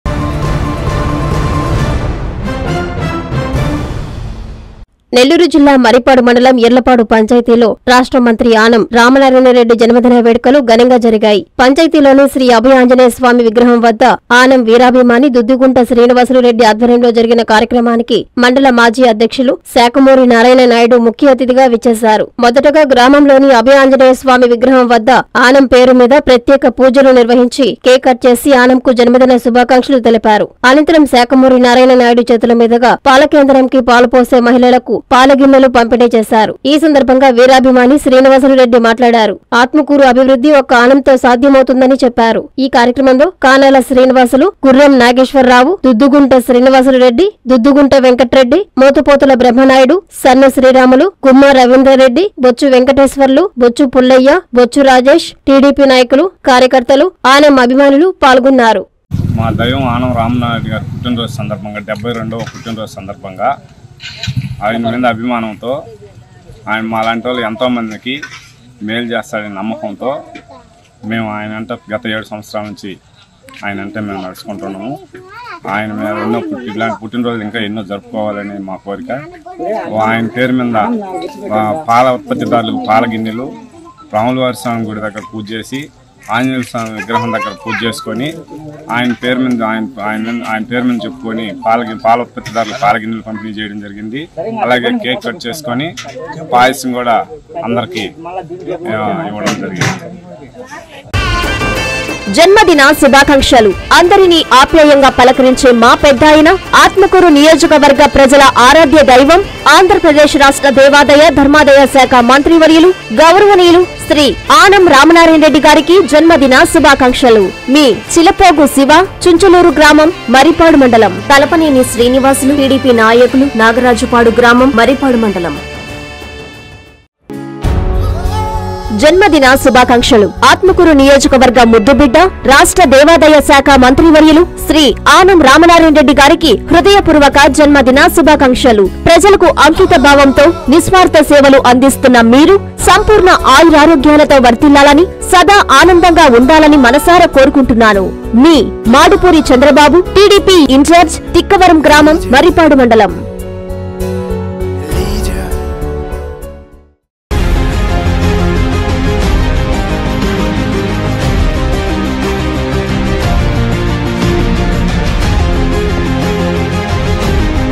మంత్రి ఆనంపై కొండంత‌ అభిమానం
- వీరాభిమాని ఆధ్వ‌ర్యంలో ఆంజ‌నేయుడుకి ప్ర‌త్యేక పూజ‌లు
- మ‌హిళ‌ల‌కు పాల గిన్నెలు పంపిణీ
- ఇర్ల‌పాడులో కేక్ క‌ట్ చేసి అంద‌రికి పంచి పెట్టిన వీరాభిమాని దుద్గుగుంట